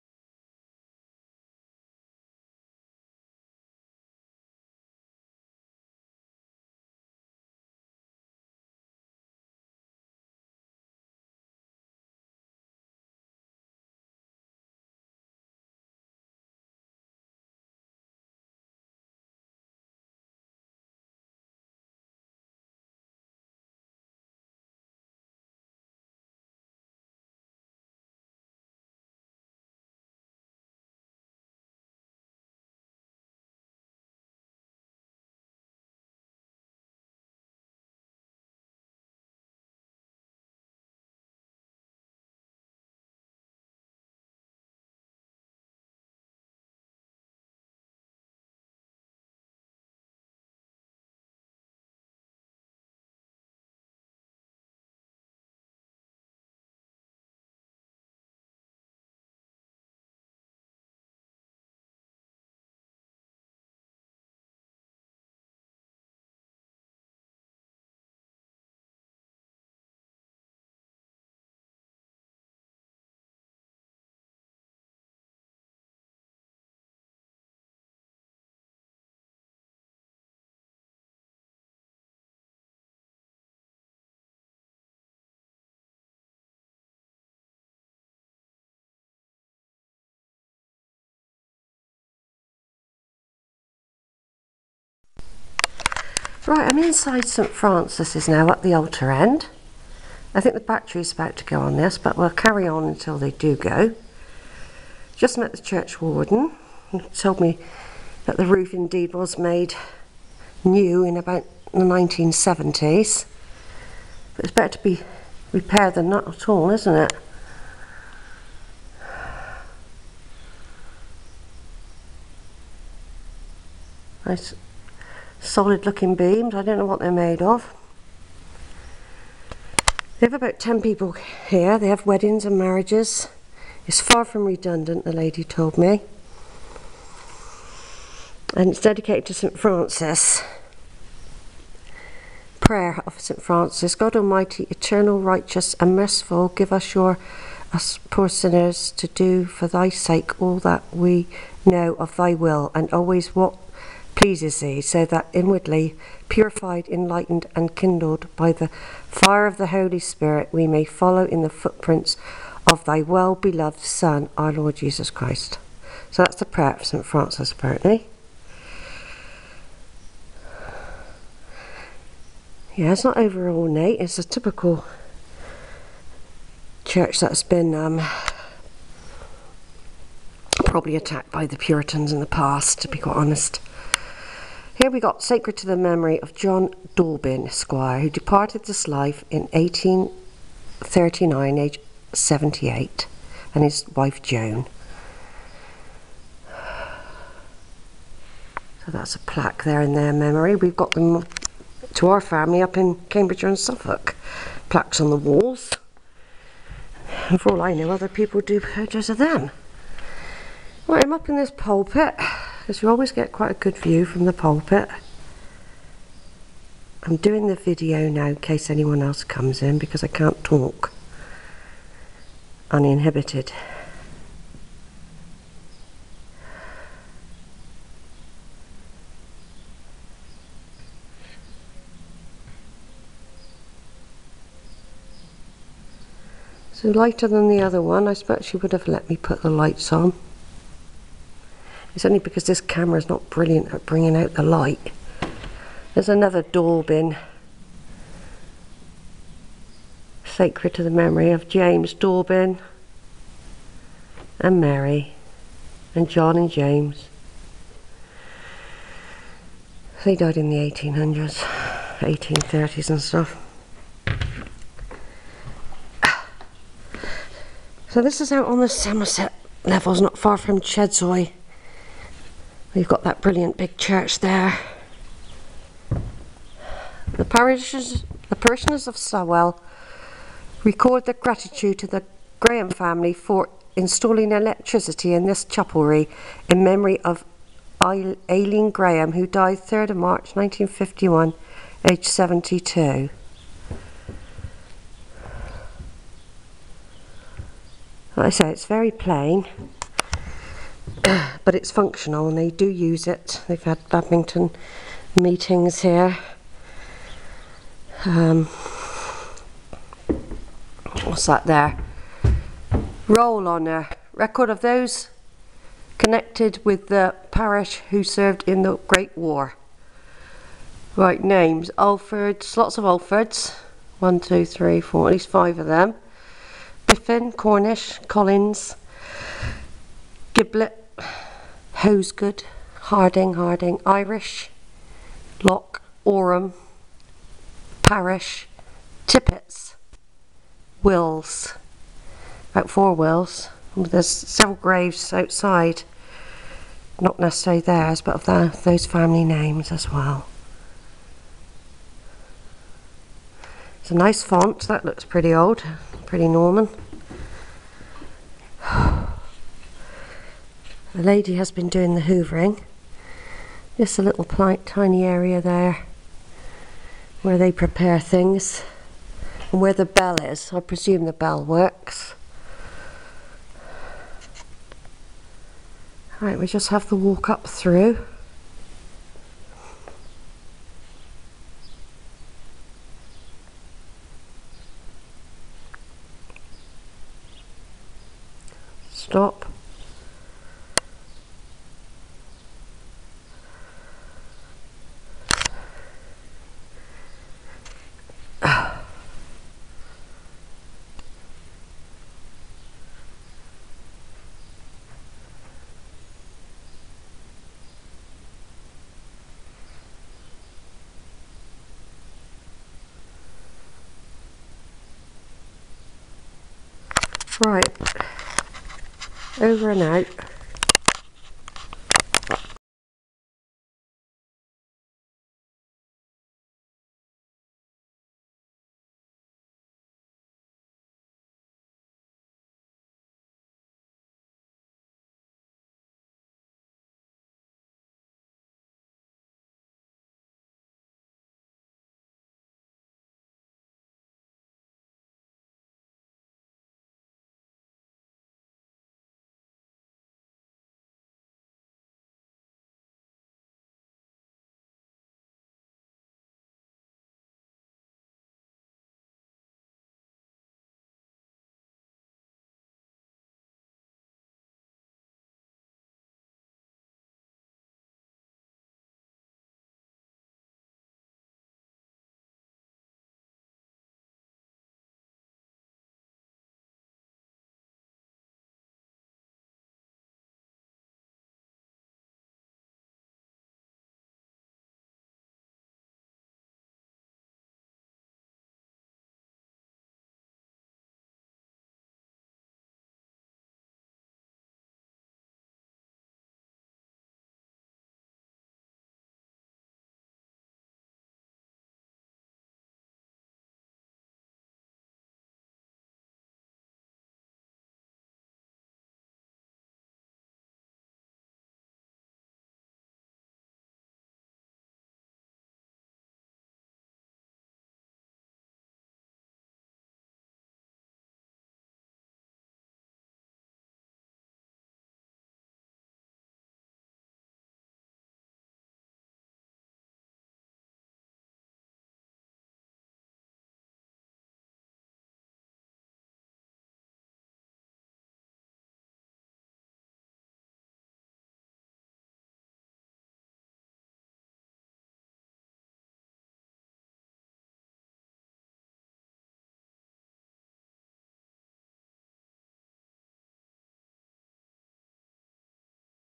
A 3 Sedgemoor Churches Cycle, 'Inside St Francis at Stawell', PART IV, July 2012 AVI
The name Stawell comes from the Saxon " a stony stream" or "well".It is part Norman--the nave built in 12th c. and chancel and north aisle in 13th c. On the right-hand side of the doorway of the porch is a cross carved within a circle--sign of crusader.The low pillars within could be that the church was once on much lower ground and has been built up to avoid flooding--they were also the archway to the north aisle now gone.Much restoration took place on1874 when the main roof was renewed and present pews and pulpit installed. I met a church helper during the visit and was informed that social events act as fund raising for the church maintenance..